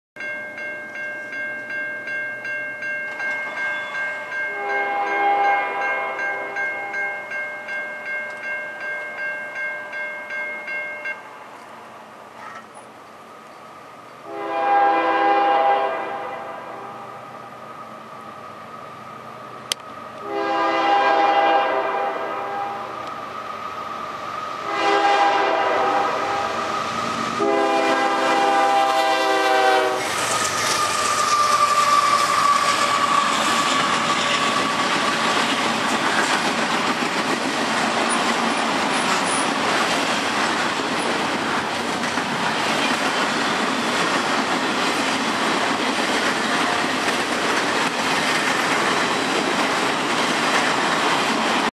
Northbound UPS Train on CSXT River Subdivision
A CSX Northbound UPS Trailer Train pulls up the Grade near Automatic Signal QR111(G).  We are at Browns Crossing Rd in Catskill NY